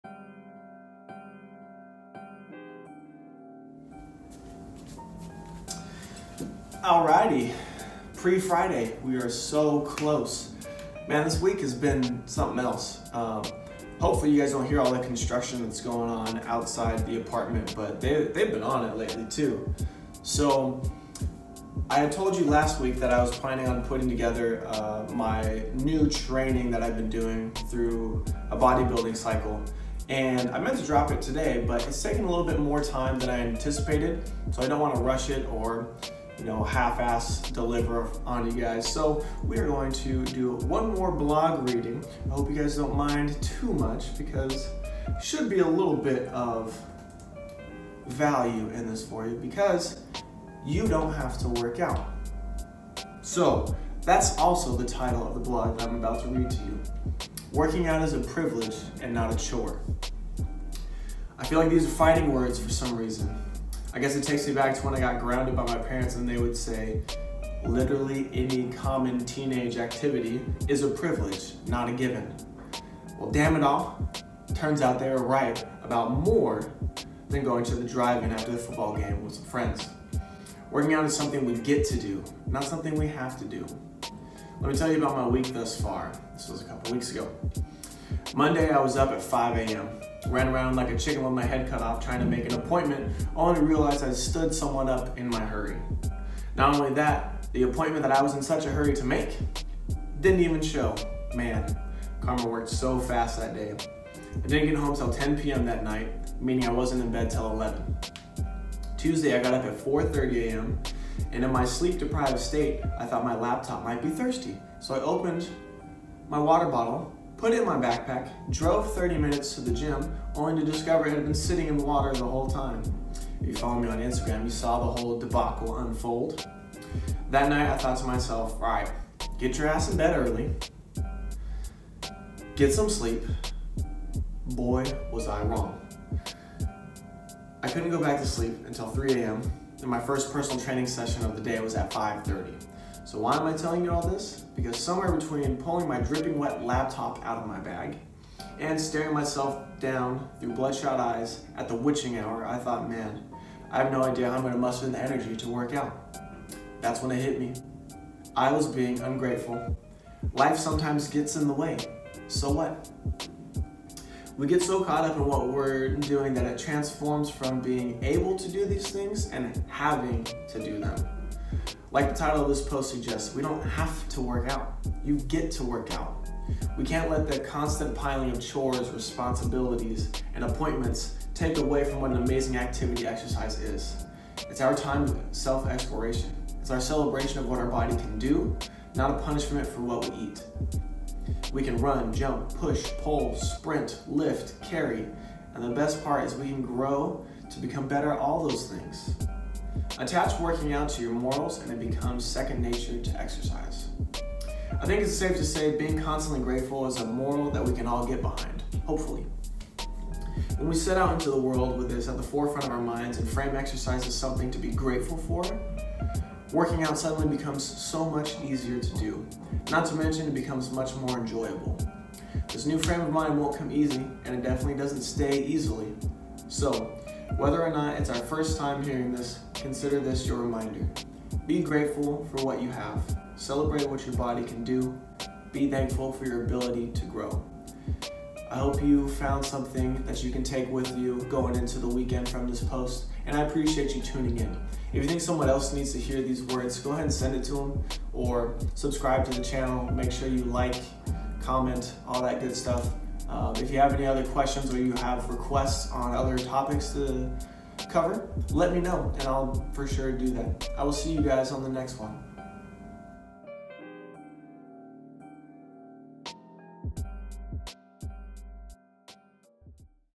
You GET To Workout
"Exercise should be a Celebration of what your Body can do. Not a punishment for what you ate."

Be grateful for your#Health. Grow your body into whatever you want it to be. Working out is a Privilege. Not a Chore.

Want to read the whole blog? Here it is!